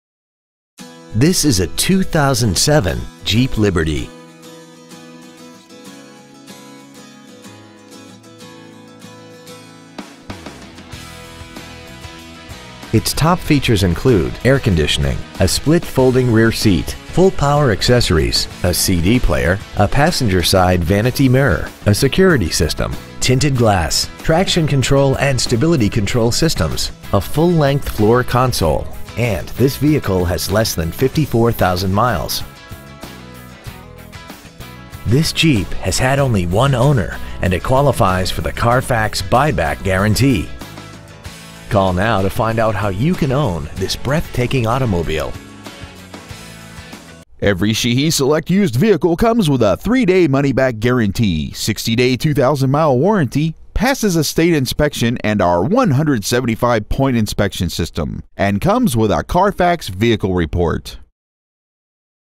Used 2007 Jeep Liberty Waldorf MD 20601
We have been honored to serve the Waldorf MD area , we promise that your experience at our dealership will exceed your expectations !

 Year : 2007
 Make : Jeep
 Model : Liberty
 Engine : Gas V6 3.7L/225
 Trans . : Manual
 Exterior : Bright Silver Metallic
 Miles : 53,556
 Interior : Medium Slate Gray
 Stock : V3578A